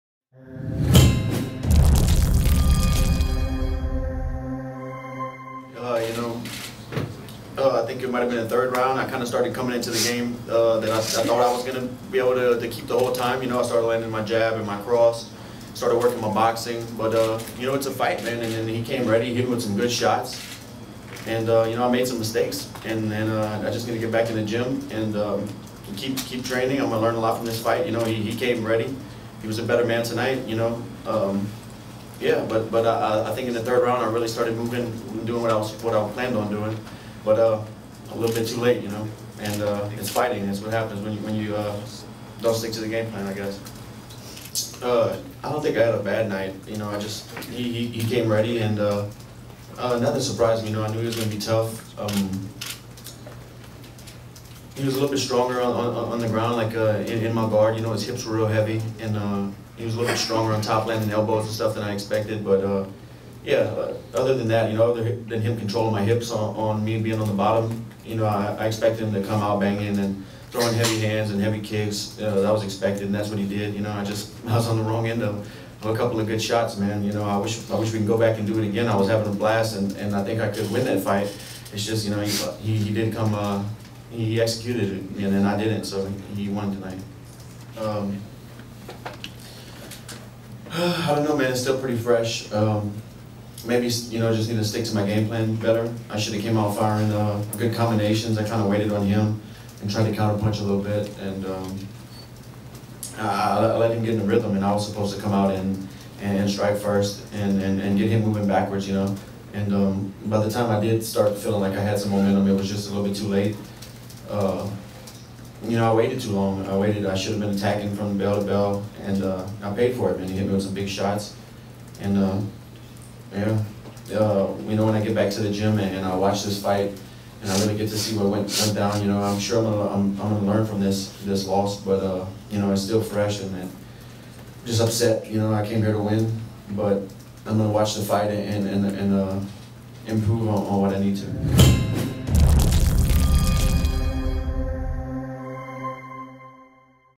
UFC on Fuel 3 Video: Lesson Learned, a Better Dustin Poirier Will Return Next Time
-- Tuesday night was not the best Dustin Poirier the world as seen, and the young featherweight contender is humble enough to admit that after only his second loss under the Zuffa banner.

Poirier lost by submission to 'The Korean Zombie' Chan Sung Jung at UFC on Fuel 3, and following the fight admitted that he didn't have his best night.

Check out what Poirier had to say about the fight, his strategy and what he learns from this loss: